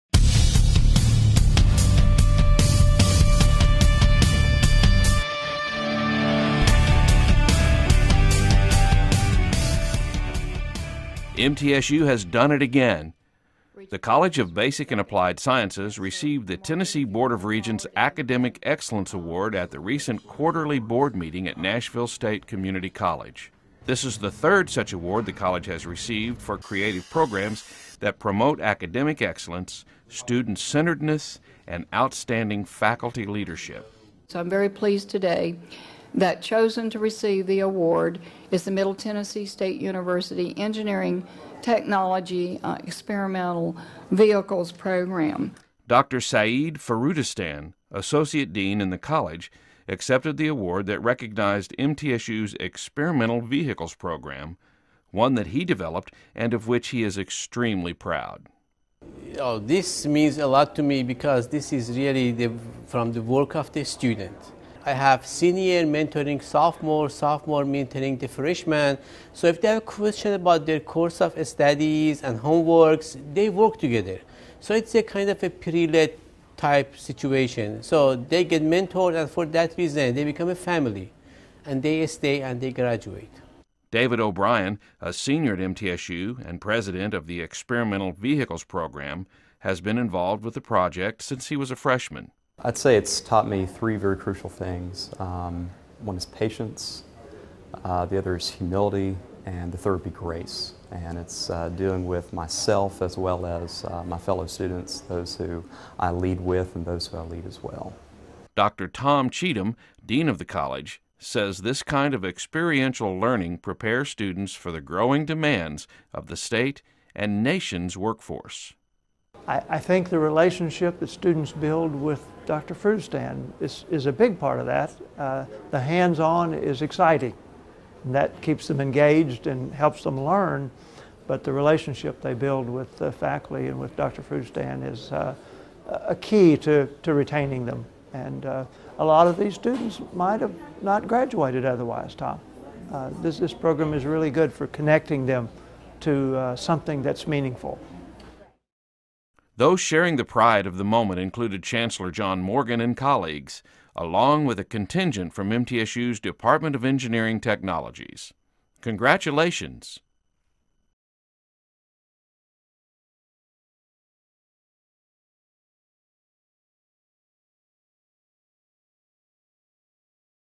MTSU Experimental Vehicles Program Wins TBR Excellence Award
MTSU's experimental vehicles program has received the Tennessee Board of Regents' Academic Excellence Award because of the program's many benefits and outstanding success.